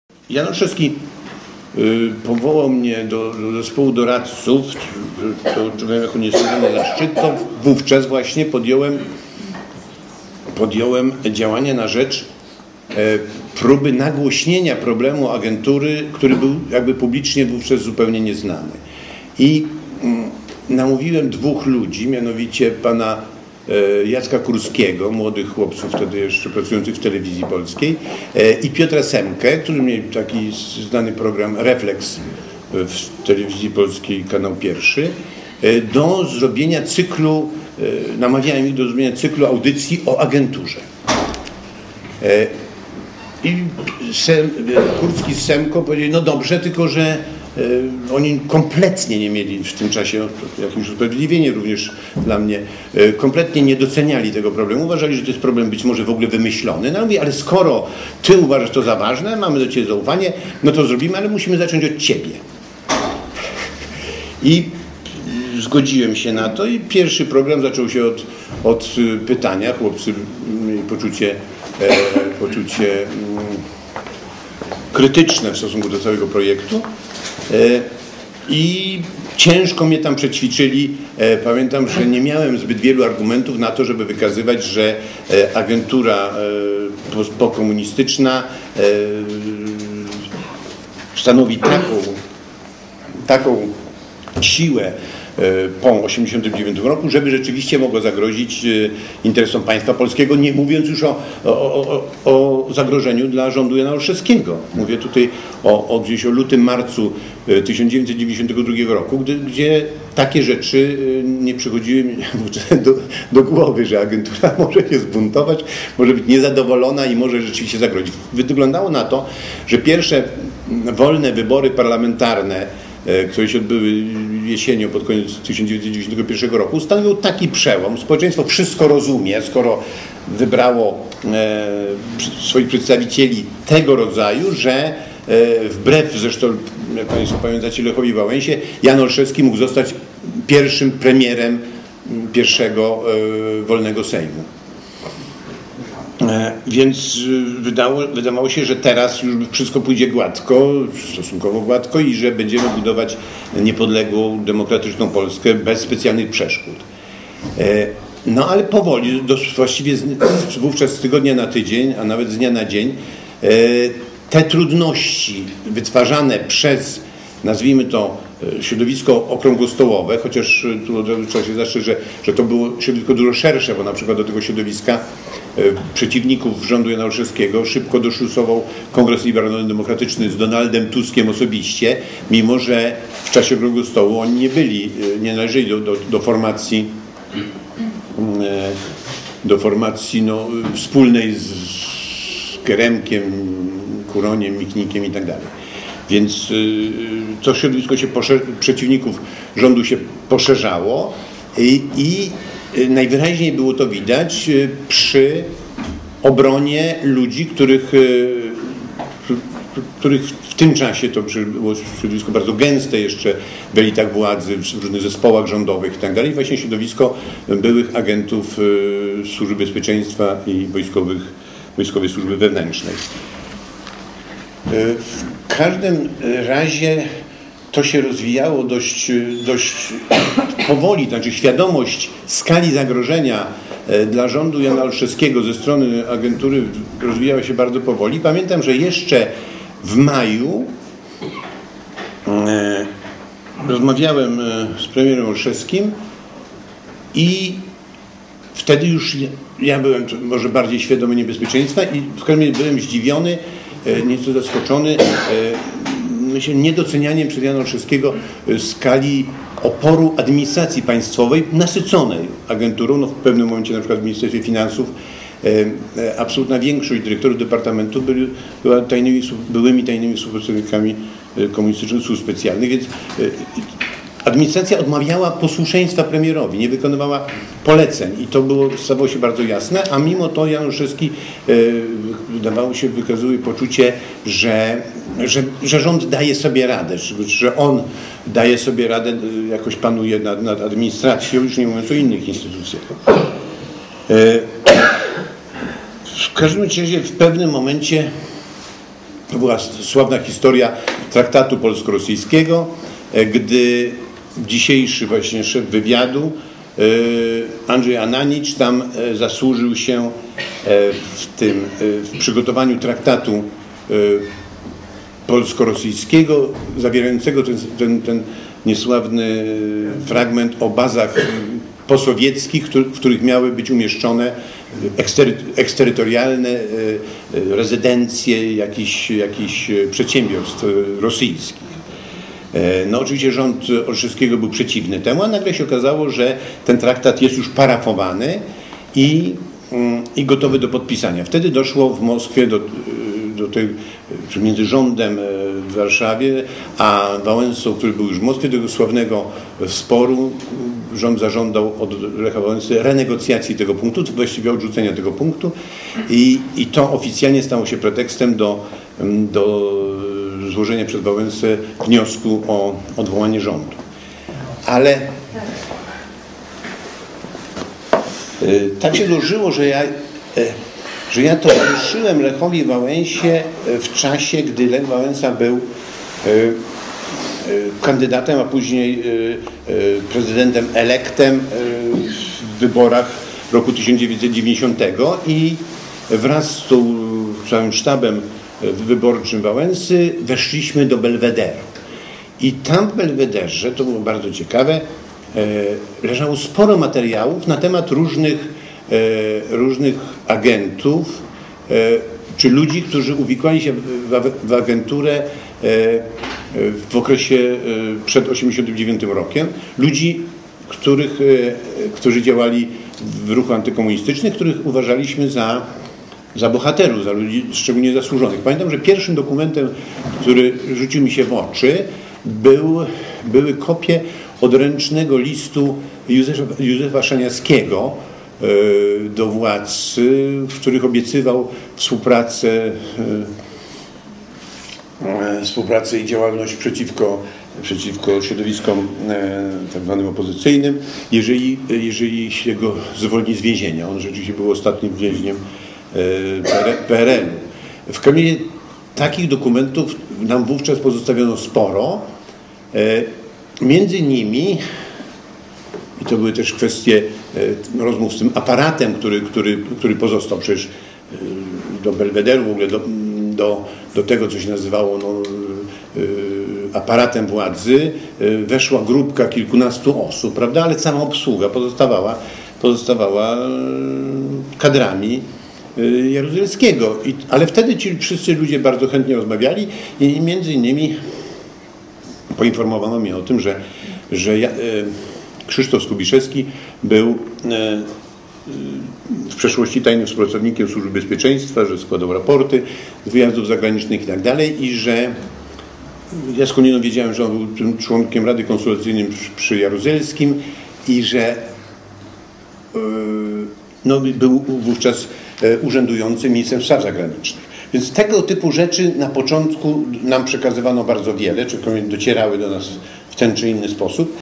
Agentura w III RP cz. 2/4
Klub "Gazety Polskiej" w Gdyni. Spotkanie z Krzysztofem Wyszkowskim. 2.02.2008. Autorem nagrania jest Pan Rafał Kobylański. Część 2/4